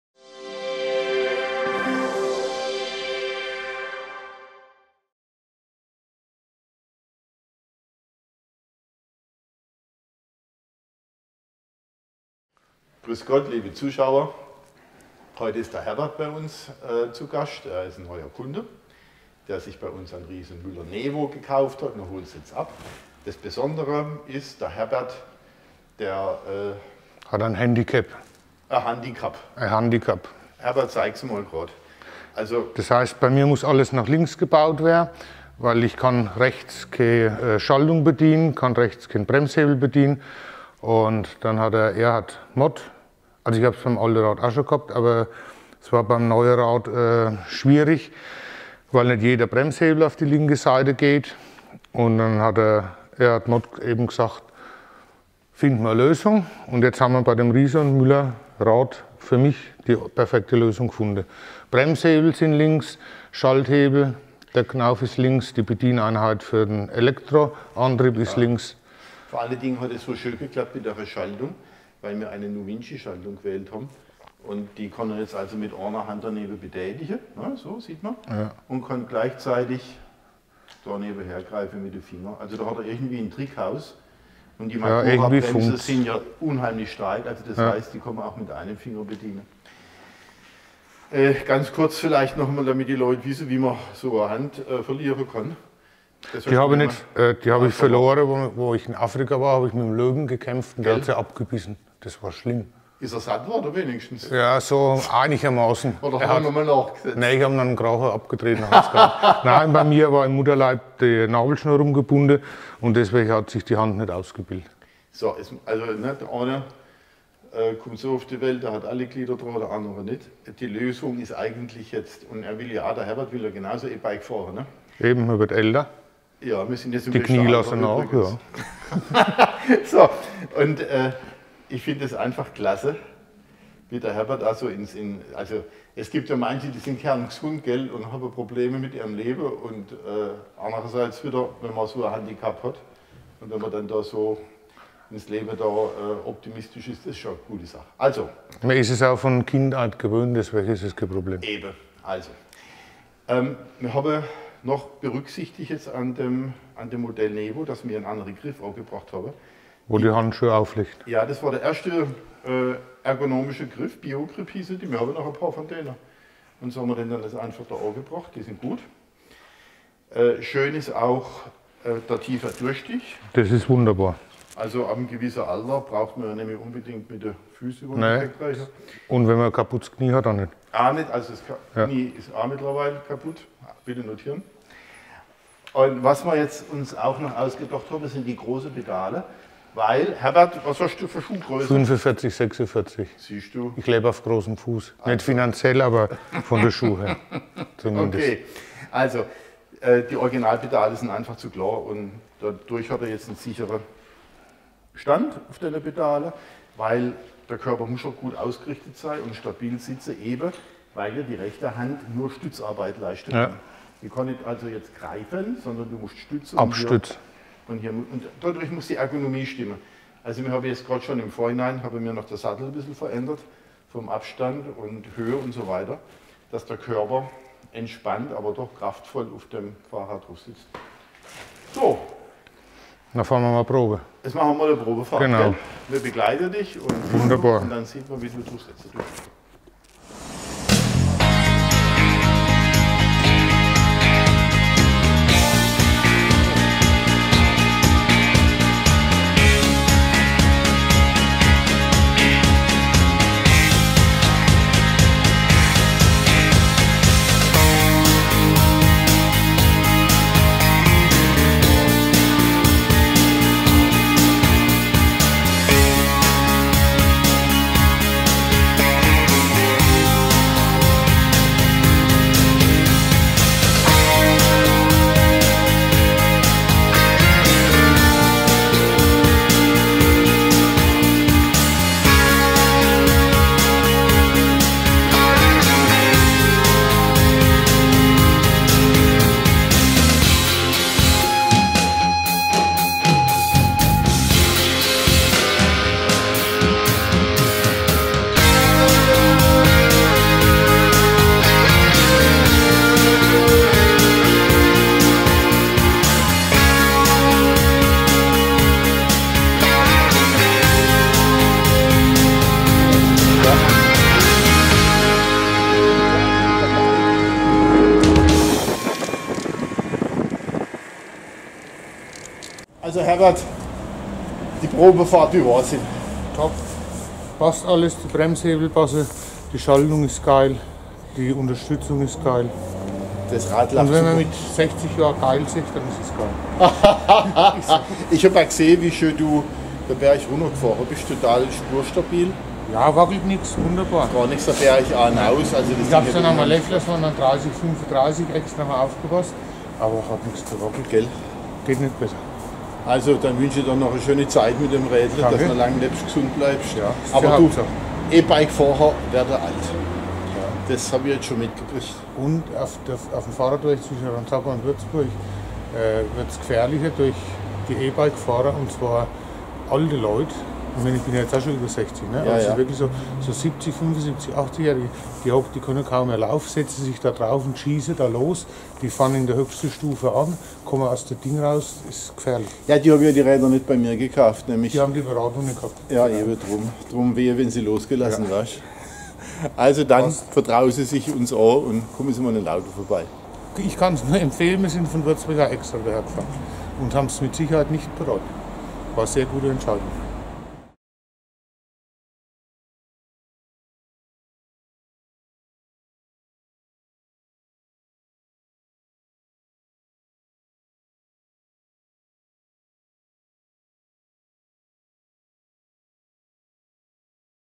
Riese & Müller Nevo ~ einhändig E-Bike fahren mit Handicap
Herbert Vornberger hat nur die linke voll funktionsfähige Hand, mit der rechten  Hand kann er sich auf dem Lenker lediglich abstützen. Trotz Handicap möchte er E-Bike einhändig fahren und hat sich für ein Riese & Müller Nevo mit NuVinci Schaltung entschieden.
Wir zeigen Ihnen in dem Video, wie wir das Riese & Müller Nevo auf Einhandbetrieb umgebaut haben.